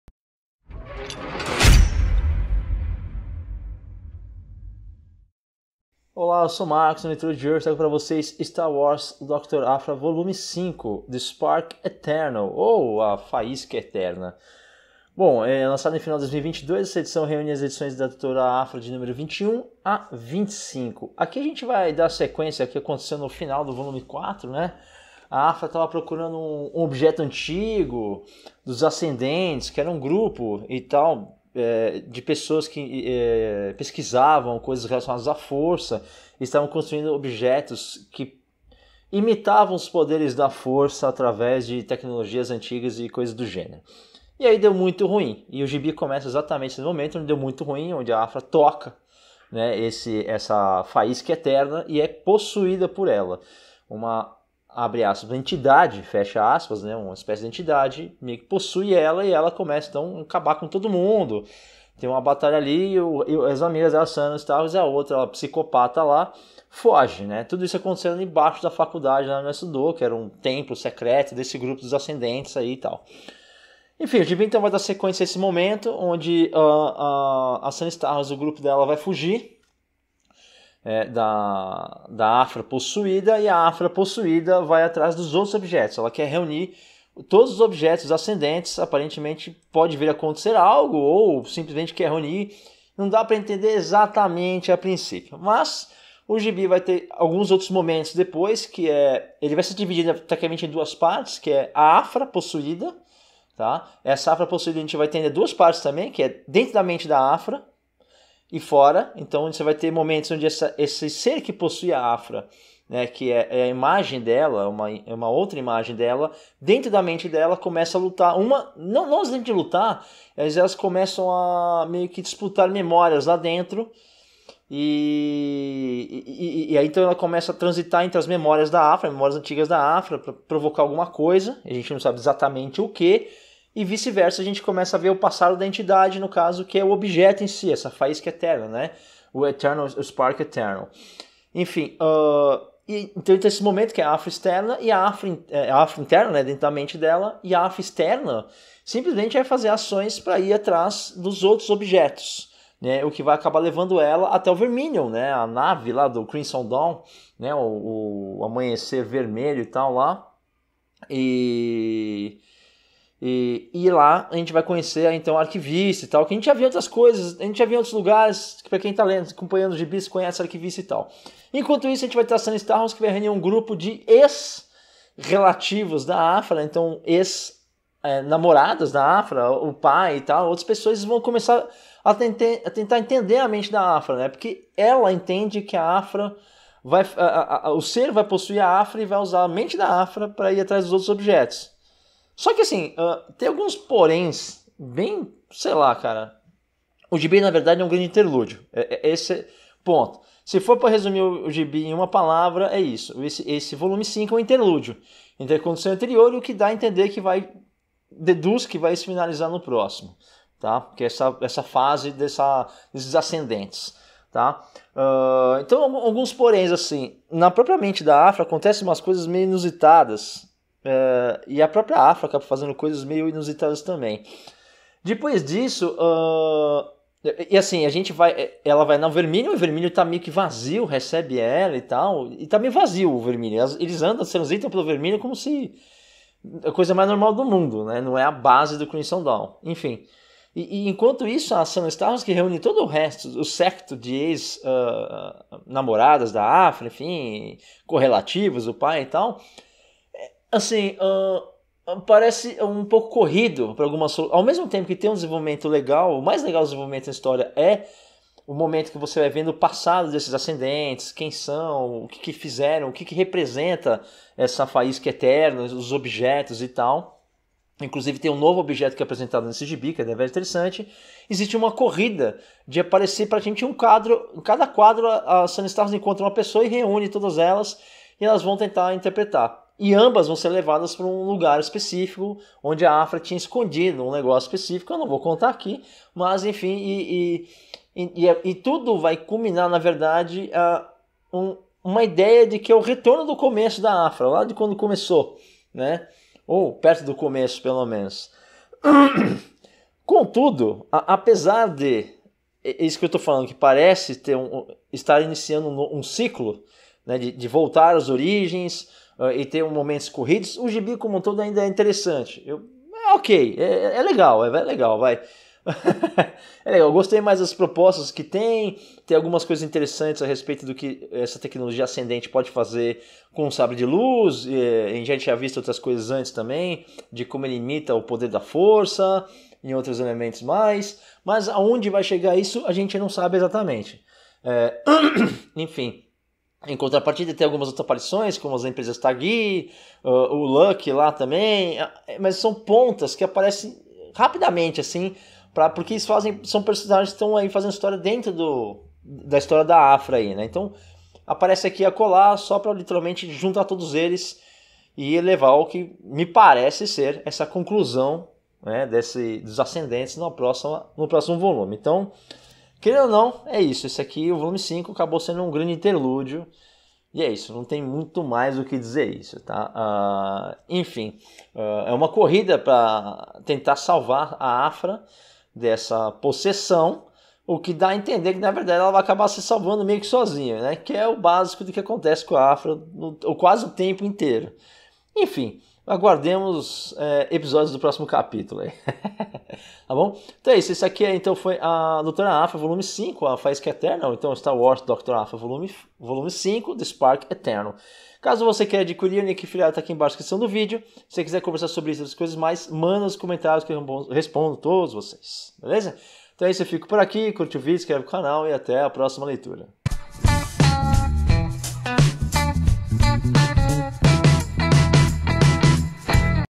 Resenha - STAR WARS: DOUTORA APHRA VOL.5 - A FAGULHA ETERNA - Leitura 513
Aphra e Sana partem rumo a um confronto que vai mudar o rumo de suas vidas! A Fagulha Eterna corrompe tudo aquilo que toca, e a doutora não é exceção!